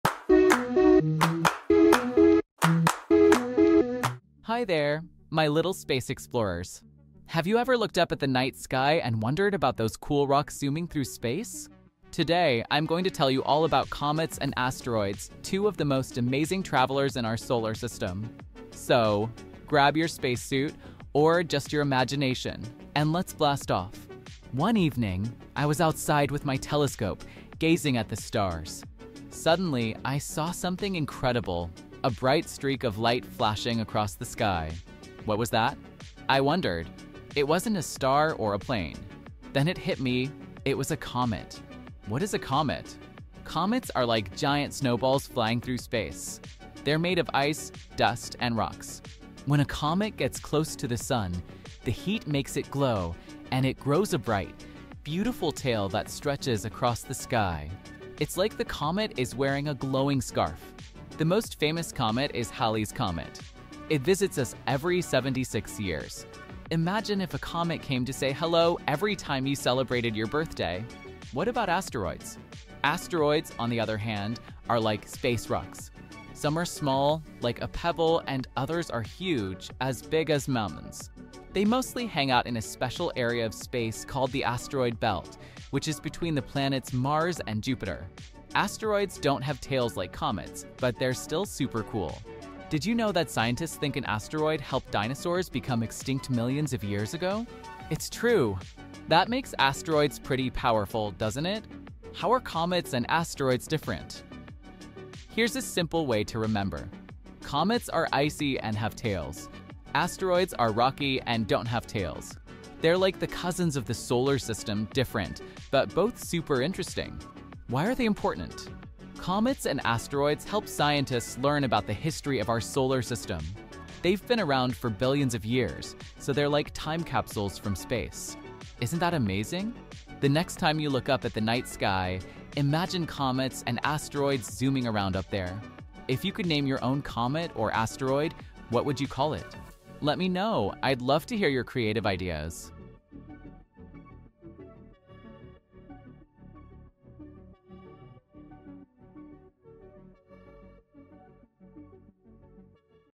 Comets vs. Asteroids – Fun Space Facts for Kids! 🚀 | Educational Videos For Kids
Hey, little space explorers! 🌙 Have you ever seen a bright streak in the sky and wondered, “What is that?” It could be a comet or an asteroid! Join me on an exciting space adventure as we learn all about these fascinating space rocks. 🚀✨

In this video, you’ll discover:
📌 What are Comets? Learn how comets shine and why they have bright, sparkling tails! 🌠
📌 What are Asteroids? Explore the rocky travelers that zoom around the solar system. 🪐
📌 Comets vs. Asteroids: We’ll compare their icy tails and rocky surfaces to see what makes them unique!
📌 Why They Matter: Find out how studying comets and asteroids helps scientists understand the history of our solar system. 🔭
📌 Fun Science Facts: Did you know that some asteroids could even hold clues to the origins of life? 🧬

Enjoyed this space adventure? Let’s keep exploring the universe together! 🌌